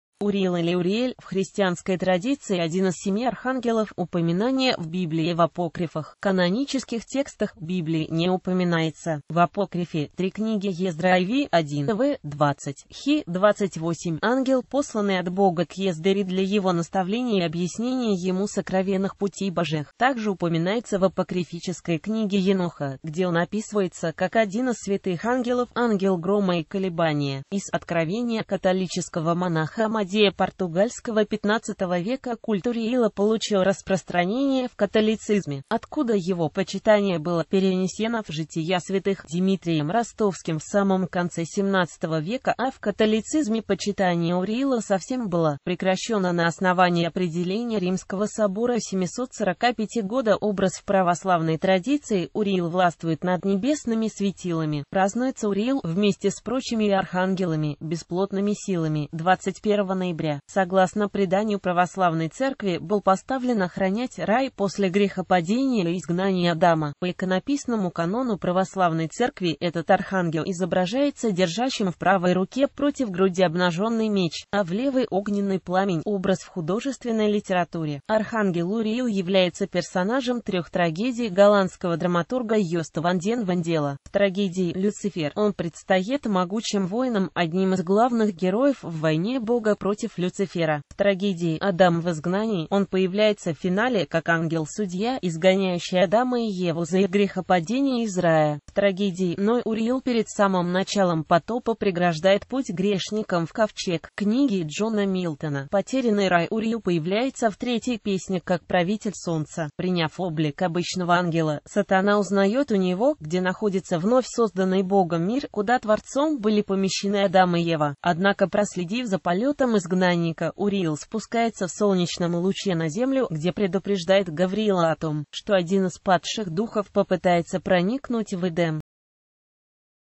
Архангел Уриил
Урии́л или Уриэль — в христианской традиции один из семи архангелов.

Профиль автора: Дионисий (1440–1502) Альтернативные имена Русский: Дионисий English: Dionisius Описание Российский художник Дата рождения/смерти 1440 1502 Нормативный контроль